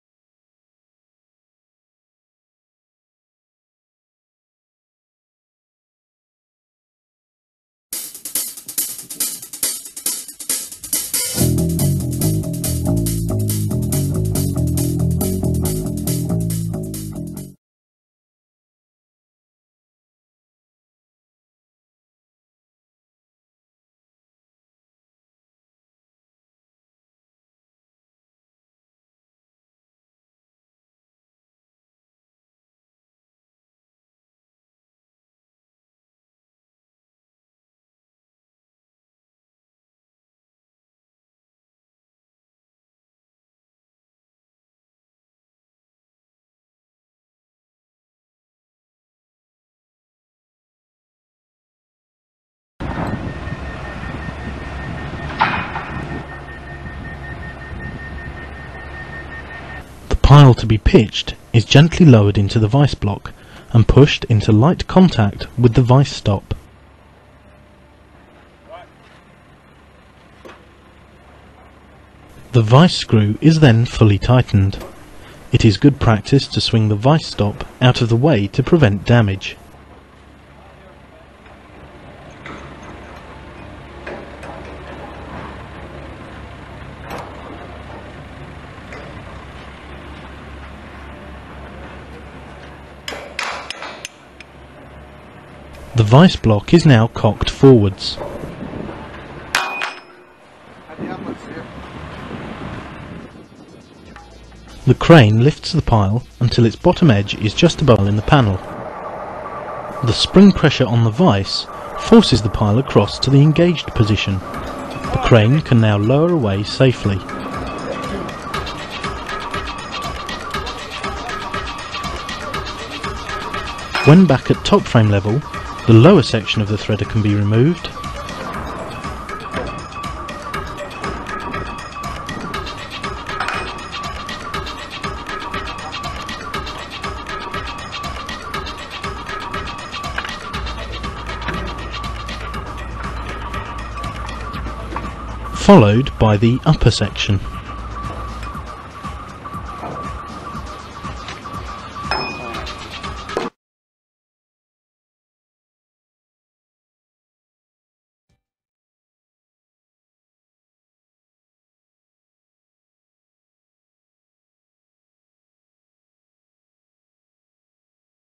Dawson - Sheet pile threaders - Old vs New
A whole new way of lowering sheet piles, the new and innovative way is much safer and secure.

For pitching sheet piles, Dawson offers the universal sheet pile threader. Advantages of this machine include that the method it uses is productive, easy and safe. All operations can be performed on top frame or ground level, which means that there is no more need for a “top man”.

The universal sheet pile threader can be set up for all Larssen and Z section sheet piles. When the machine is set up correctly, one simple movement interlocks the piles. Dawson advises to spend a lot of time on setting the machine up correctly to make sure that each adjustment is accurately set. The extra time spent will definitely pay off as the procedure is optimized.

Not every interlock is necessarily the same on one pile. For instance, on “Z” section interlocks, the end is usually different. In addition, the leading of the pile can be right or left hand. Using simple adjustments of roller positions, the universal sheet pile threader can accommodate these variations. These adjustments can be done on site.

For more information about the Dawson sheet pile threaders, or about other products from Dawson, contact wequips at , send an e-mail to or take a look at our website;